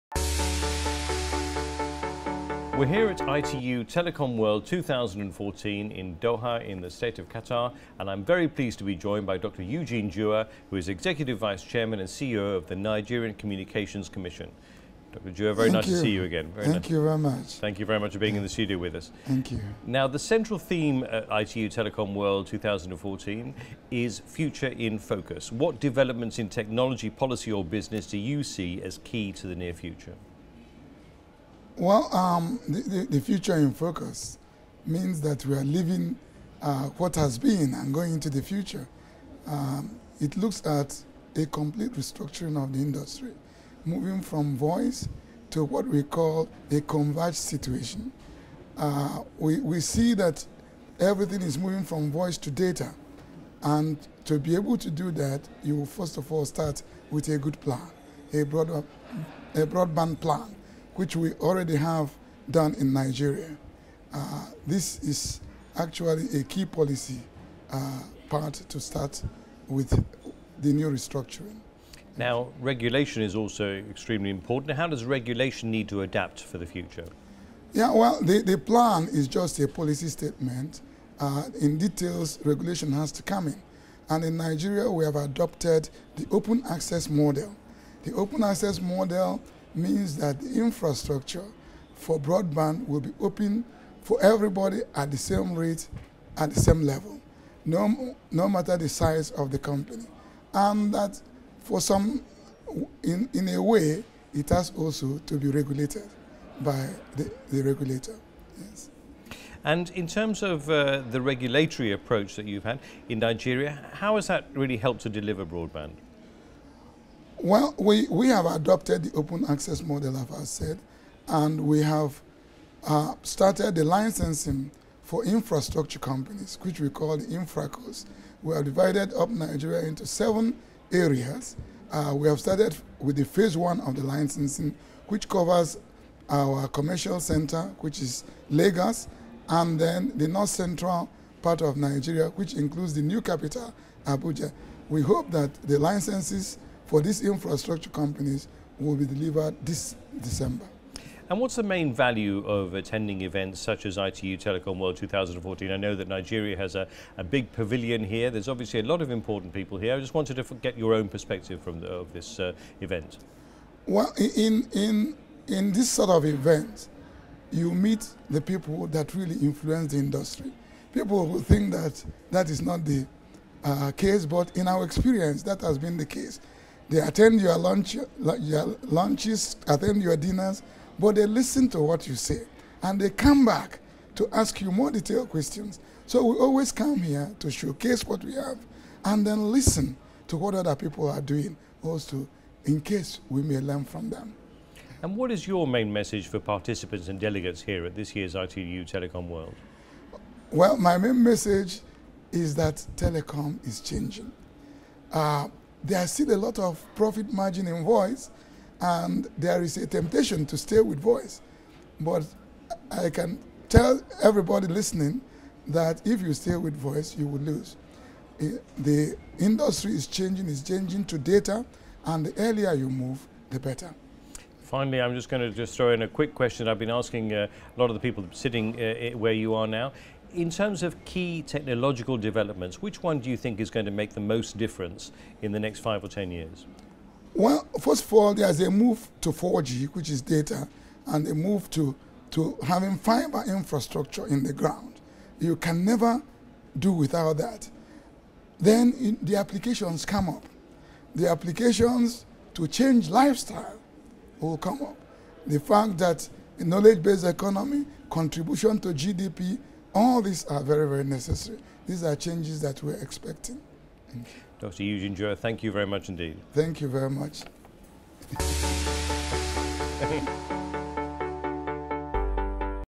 ITU TELECOM WORLD 2014 INTERVIEW: Dr Eugene Juwah, CEO, Nigerian Communications Commission
Interview with Dr Eugene Juwah, Executive Chairman & CEO, Nigerian Communications Commission, at ITU TELECOM WORLD 2014.